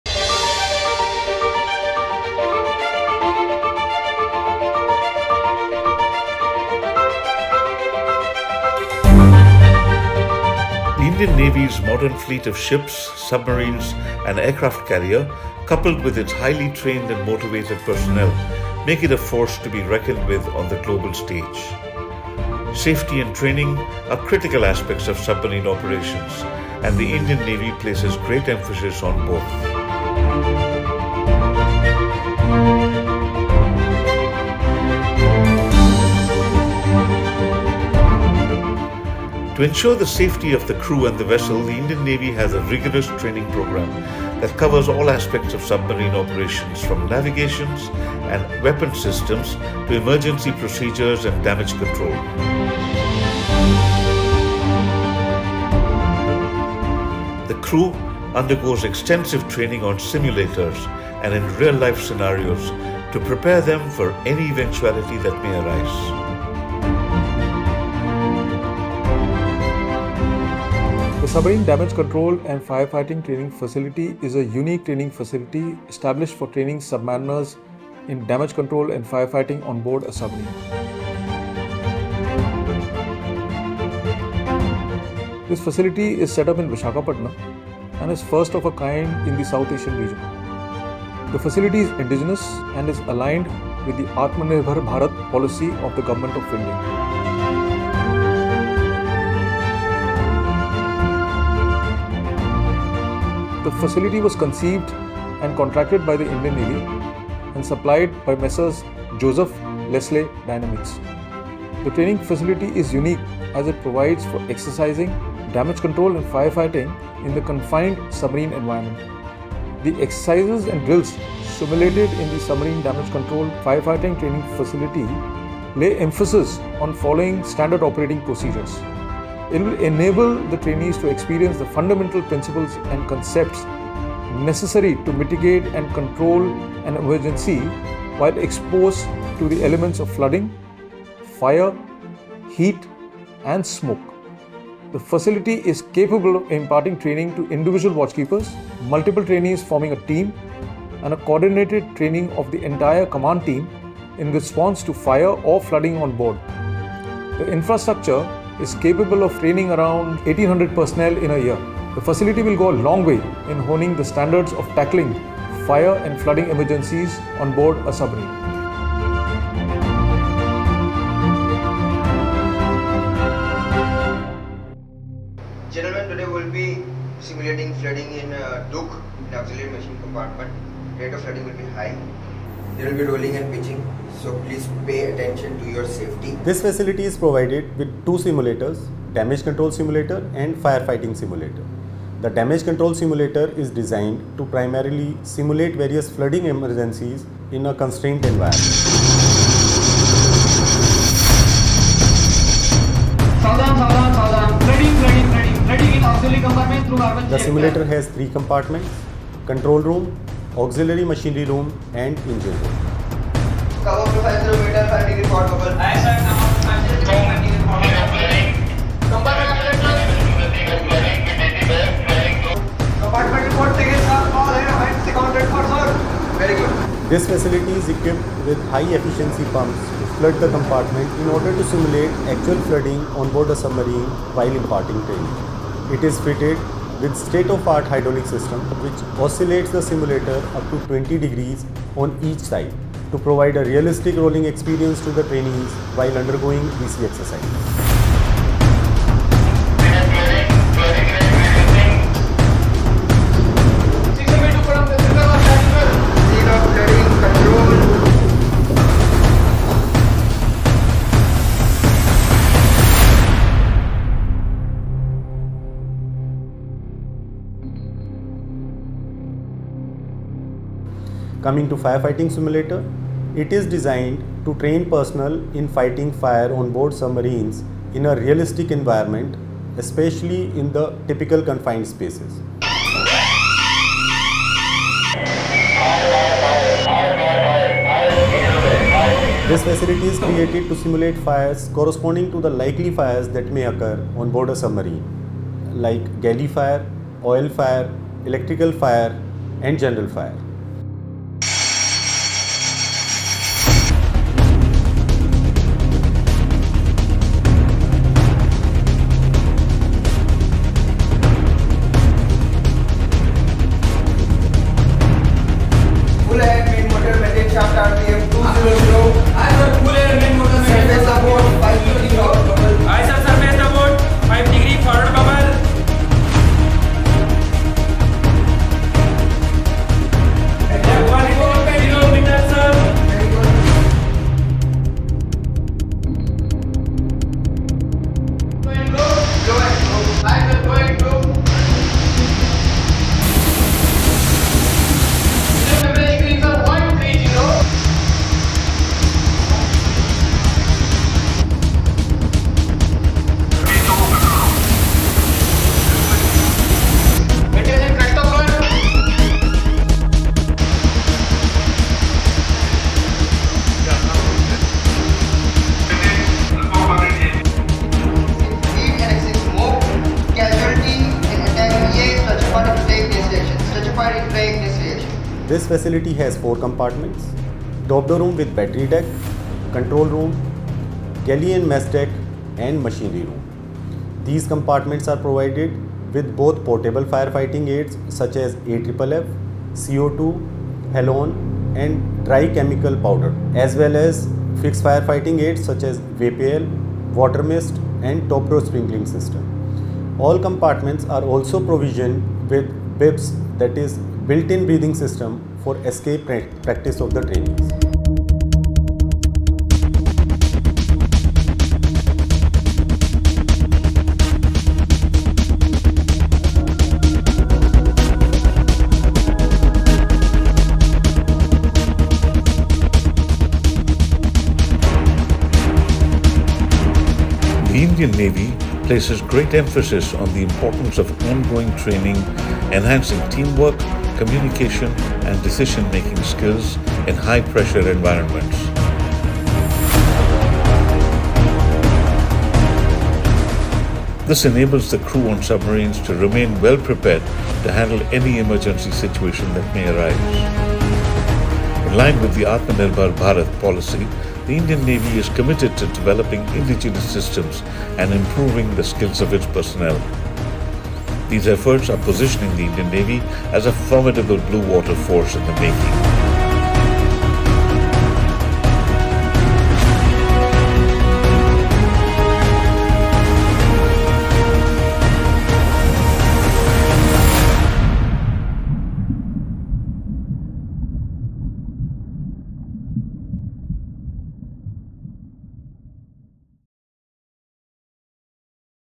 SUBMARINE DAMAGE CONTROL AND FIRE FIGHTING TRAINING FACILITY (SDCFFTF) - INS SATAVAHANA
A fire or a flooding onboard a submarine can be catastrophic. However, the risk of a fire and flooding is omnipresent onboard. Therefore the submarine crew needs to be trained to a very high standard in combatting fire and flooding onboard a submarine. SDCFFTF is a first of its kind facility being established for training Submarine personnel in DamageControl and FireFighting in the confined space of a submarine. This facility has been created indigenously and is aligned with Atmanirbhar policy of Gol. This facility was conceived and contracted by IndianNavy and delivered by M/S Joseph Leslie Dynamiks. The facility has numerous safety features with the ability to control the exercises both from Local and Main Control Room. In the simulator the trainees will be undertaking all the requisite preventive, corrective and mitigating actions without actual onboard equipment and systems getting exploited. The trainees will also be exposed to the cause and effect in a more realistic environment and add to training effectiveness. It will also enable the trainees to experience the fundamental principles and concepts necessary to mitigate and control an emergency whilst exposed to the elements of flooding, fire, heat and smoke while submerged underwater. This facility is capable of imparting training to individual watch-keepers, multiple trainees forming a team as well as coordinated training of entire Command team.